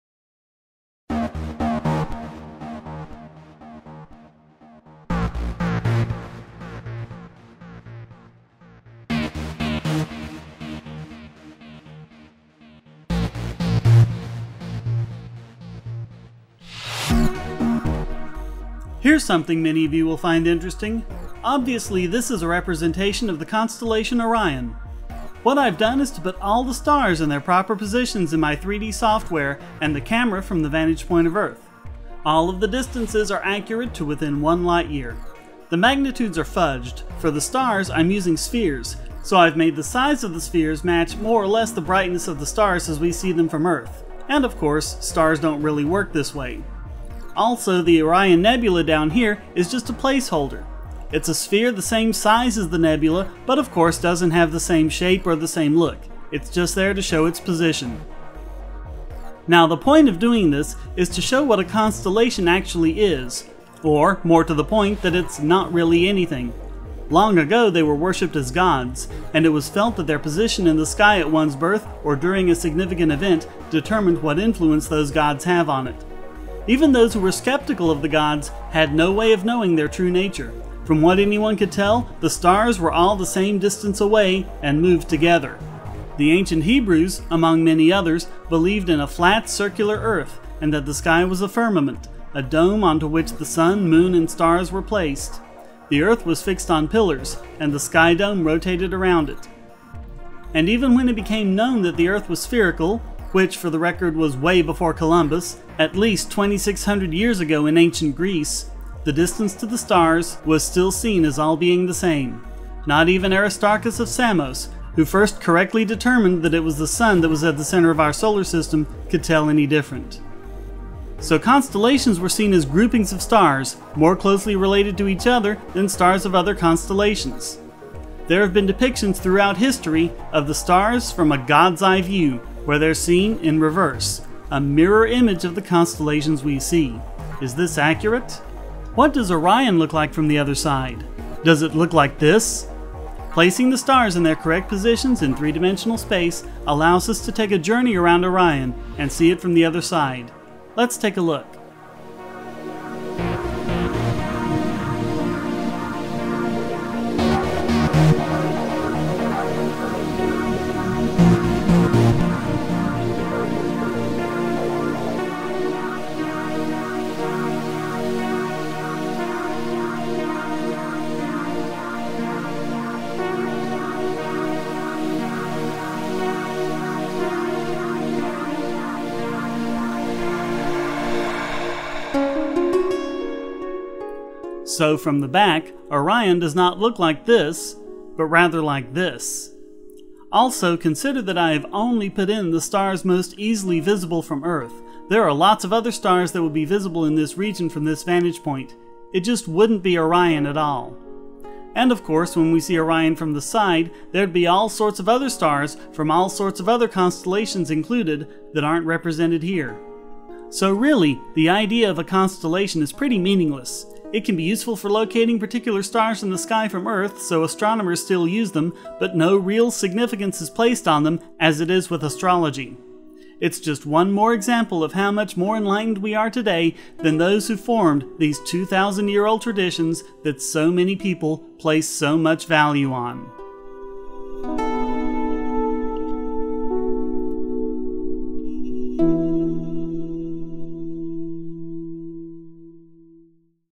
A Trip Around Orion
What exactly are the constellations? What would a constellation like Orion look like from the back? Or from the side?

This video is, as the Brits would say, "exactly what it says on the tin." A computer animation takes us 360 degrees around Orion, and shows us what a constellation is--or, more importantly, isn't.